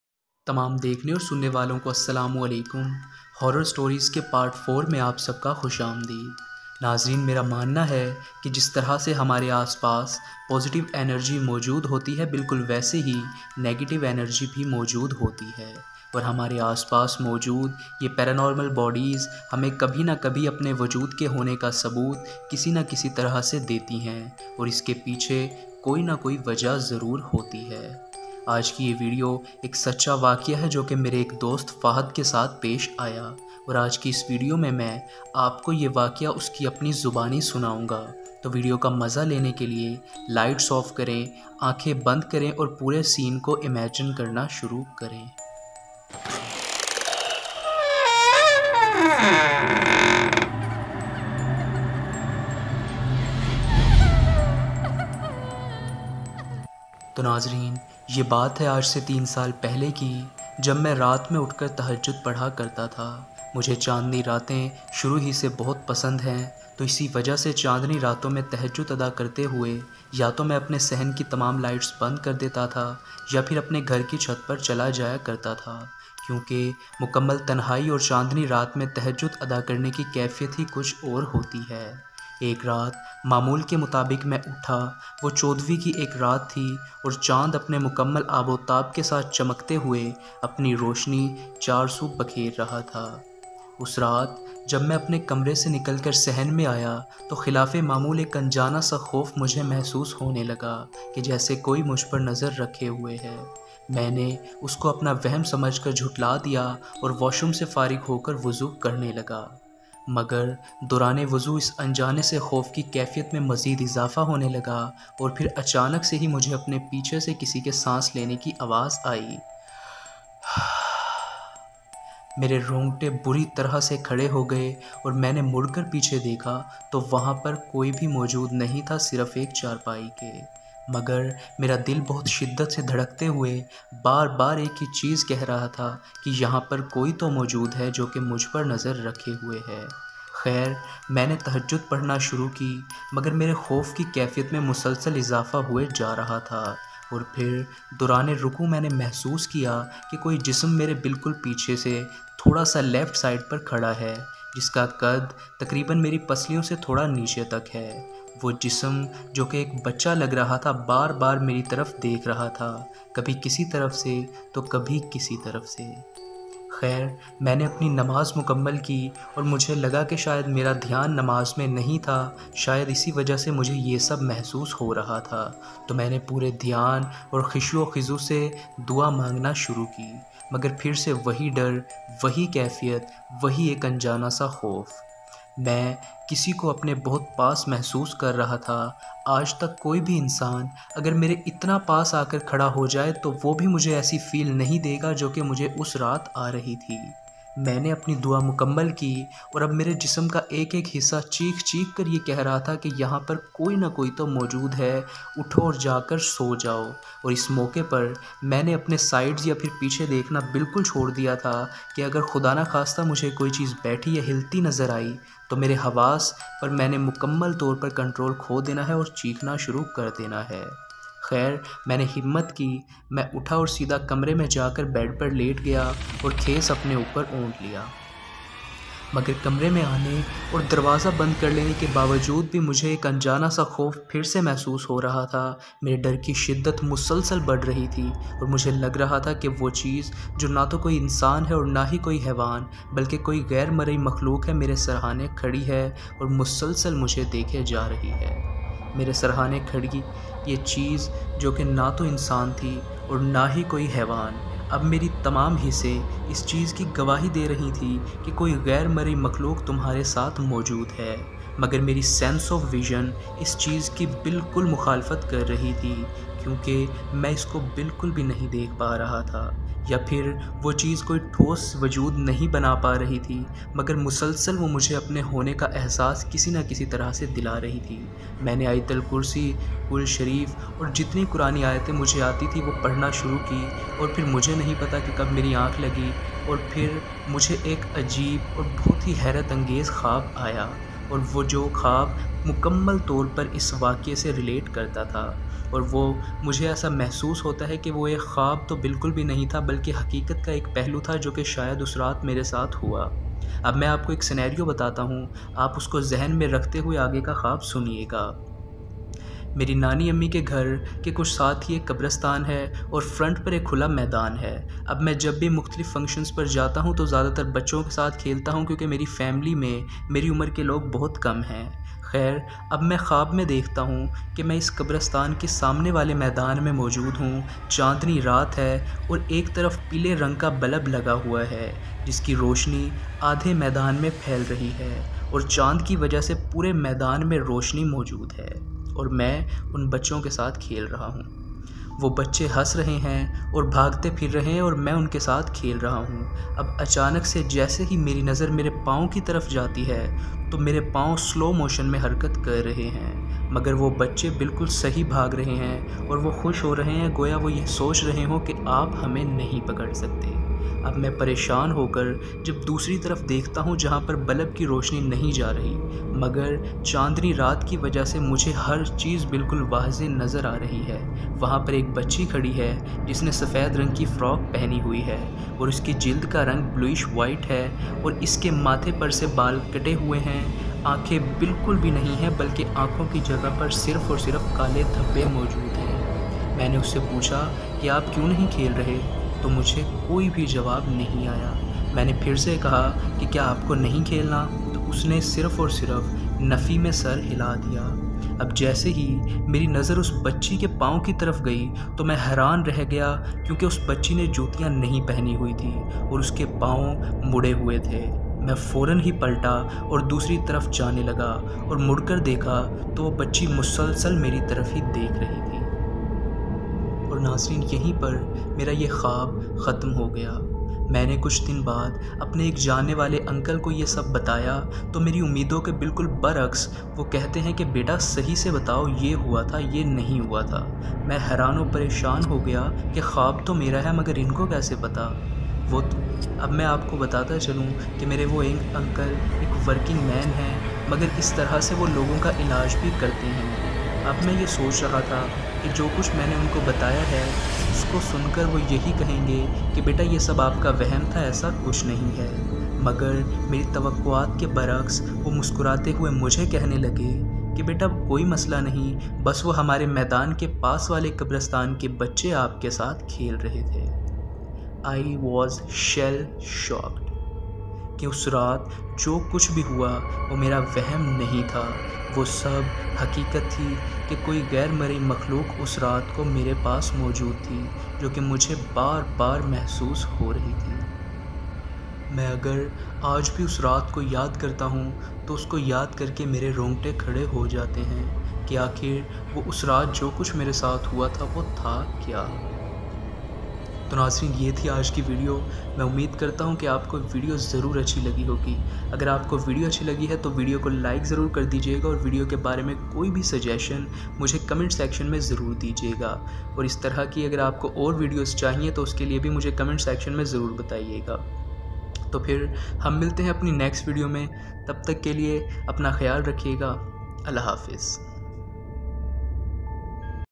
Wo Aik Raat - A Real Scary Incident With Paranormal Being | Horror Stories Part 04 | Myth TV
Hello viewers and listeners .
In this video of today, I have told you guys a horror incident happened with one of my friends . it was a spooky incident that left my friend terrified .

Watch this video to know about the complete story that how he witnessed a paranormal activity .